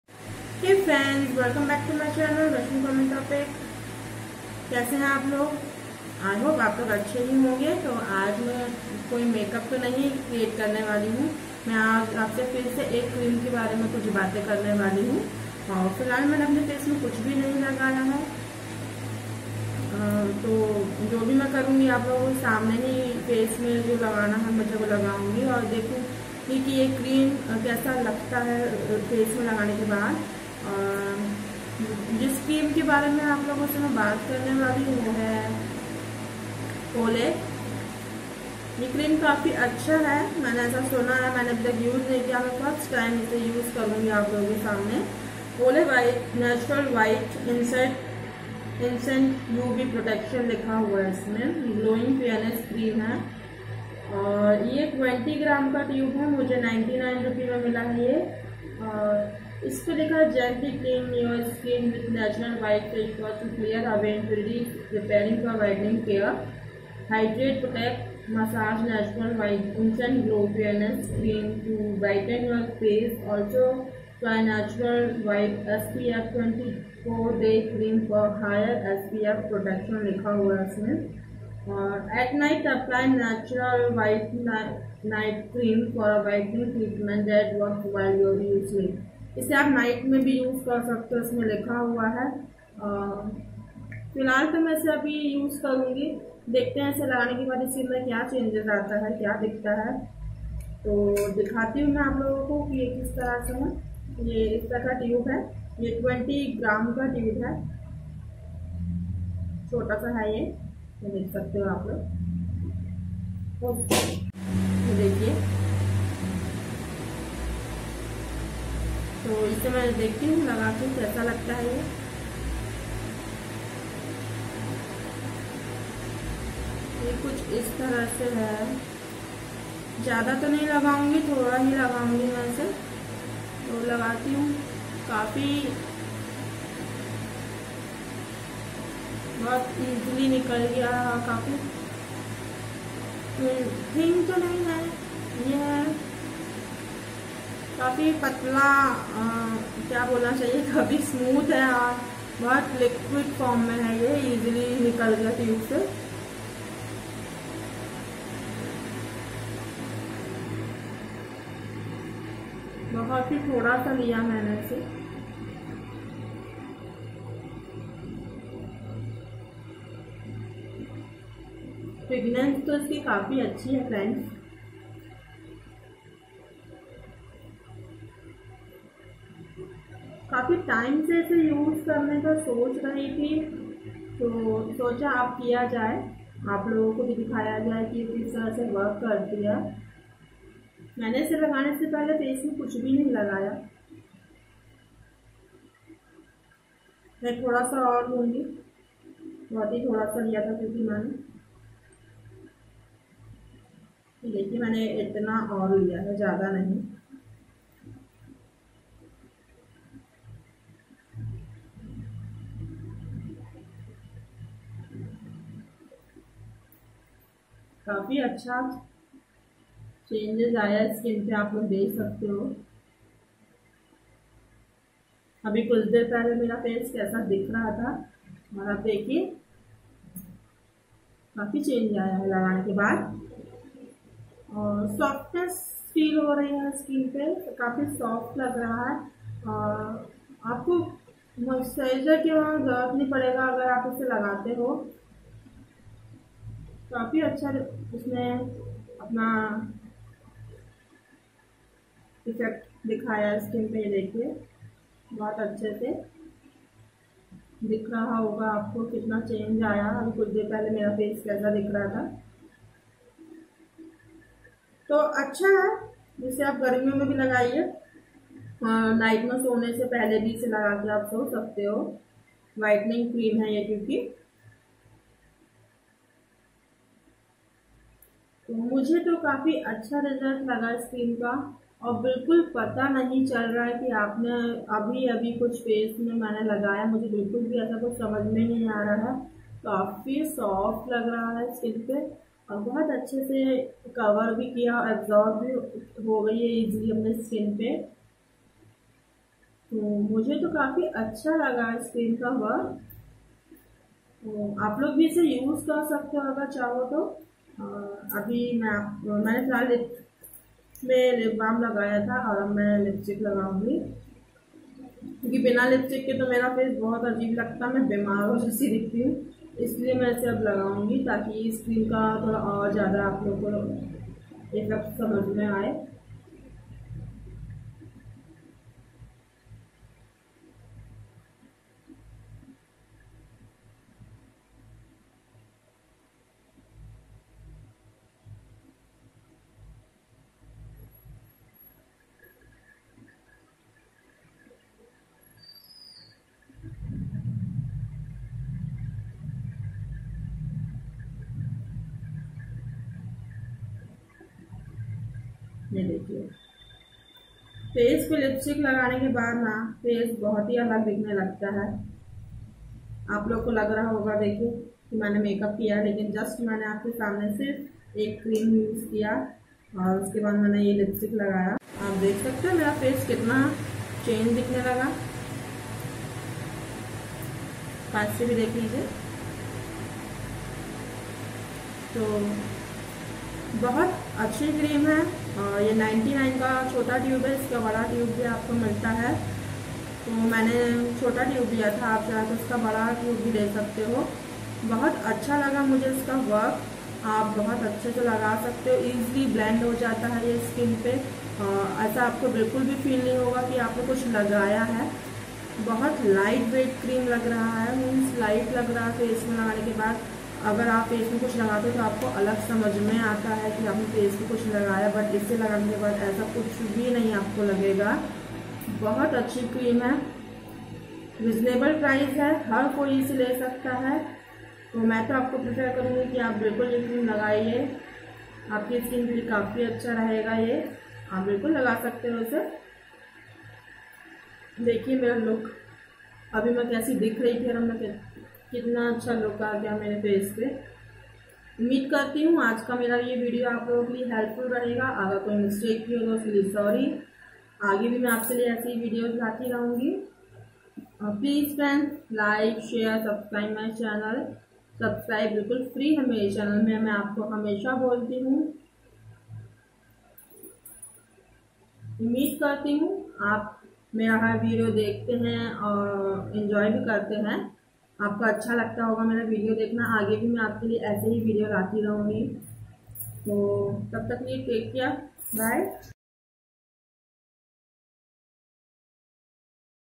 OlAY Natural White Cream ## Amazing Work
OlAY Natural White Cream

Rashmi Common Topics.

About video- Today I Use Only OlAY Natural Cream .

I Hope U All Like My Video.

Friends Luv U All.